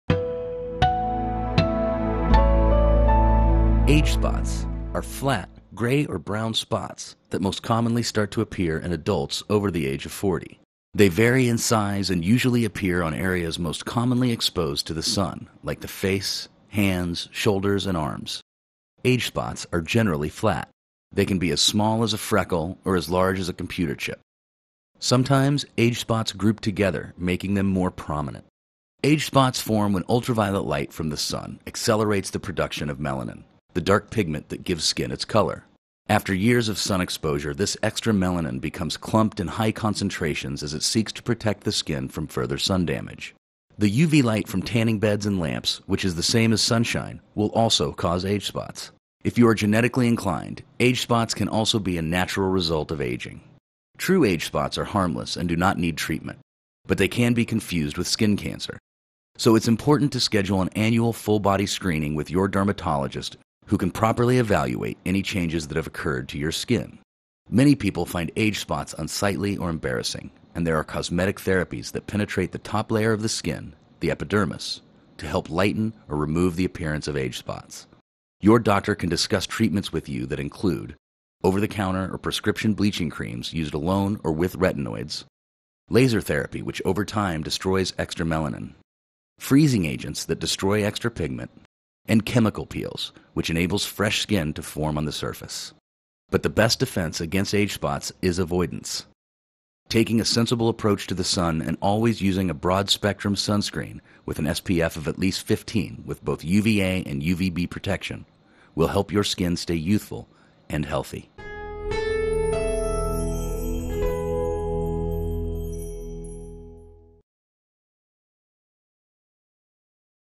Age Spots
Learn more about the causes and treatment options for age spots, also known as liver spots.